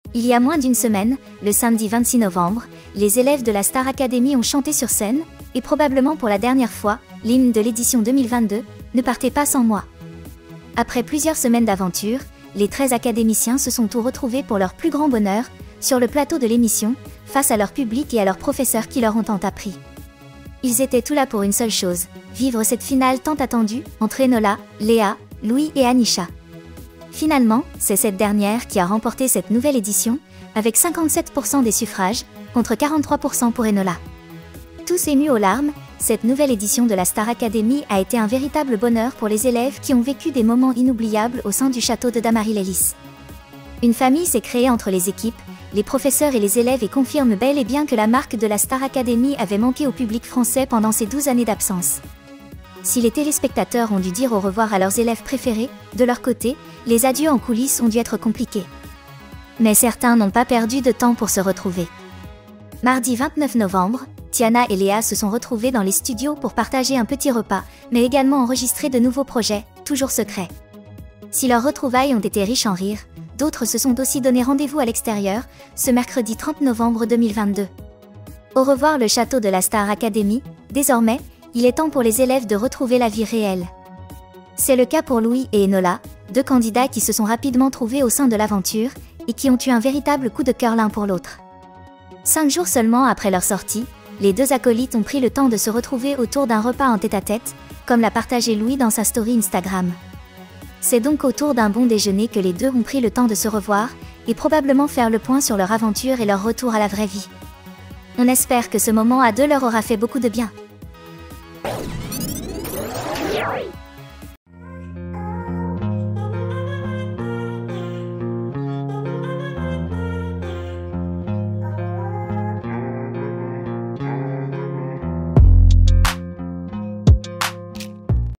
Louis partage déjà ses retrouvailles avec son acolyte Enola
Les élèves de la promotion 2022 de la Star Academy ont quitté le célèbre château pour retrouver leur petit train de vie. Ce mercredi 30 novembre, Louis et Enola se sont organisés des retrouvailles en tête-à-tête au restaurant, quelques jours seulement après la finale.